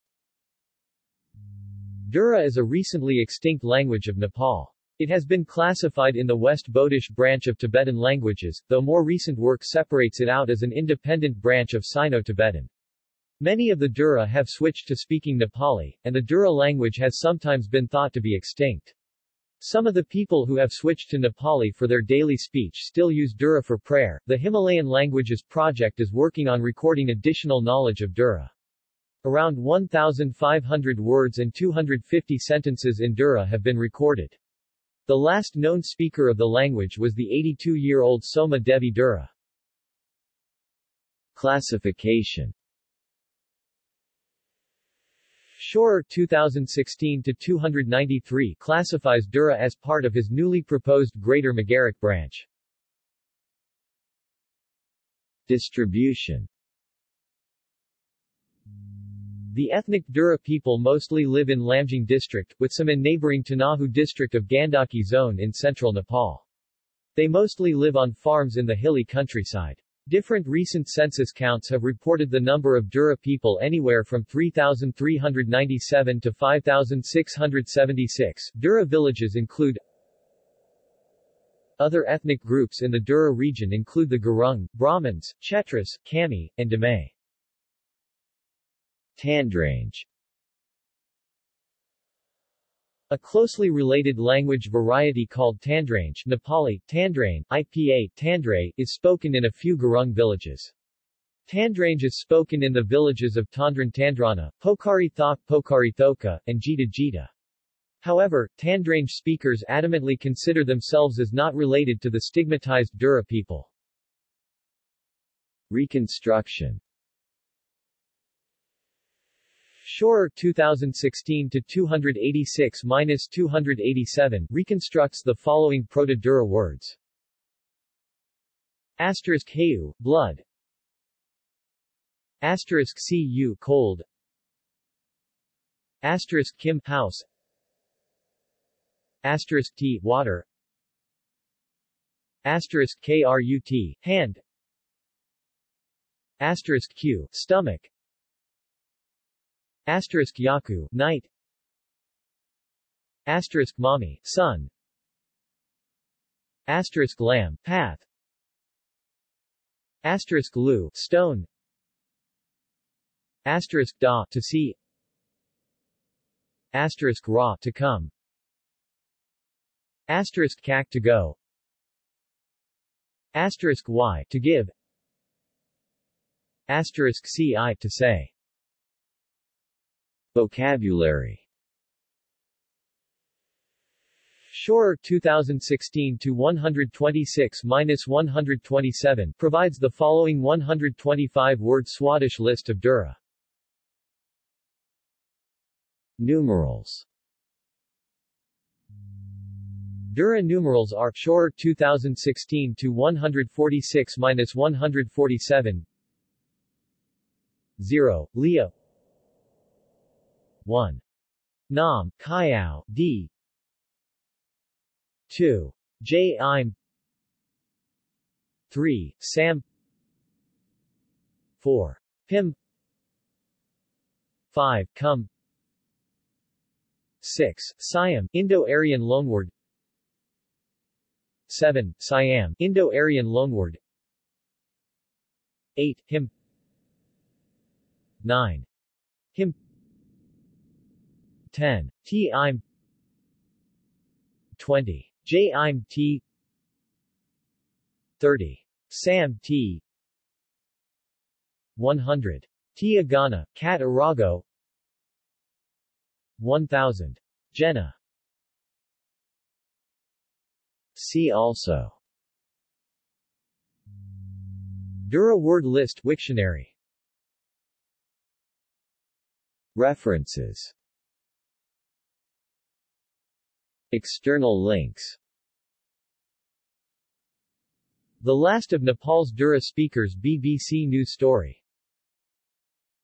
Dura language | Wikipedia audio article
This is an audio version of the Wikipedia Article:
Dura language

SUMMARY
Dura is a recently extinct language of Nepal. It has been classified in the West Bodish branch of Tibetan languages, though more recent work separates it out as an independent branch of Sino-Tibetan. Many of the Dura have switched to speaking Nepali, and the Dura language has sometimes been thought to be extinct. Some of the people who have switched to Nepali for their daily speech still use Dura for prayer.The Himalayan Languages Project is working on recording additional knowledge of Dura.   Around 1,500 words and 250 sentences in Dura have been recorded.  The last known speaker of the language was the 82-year-old Soma Devi Dura.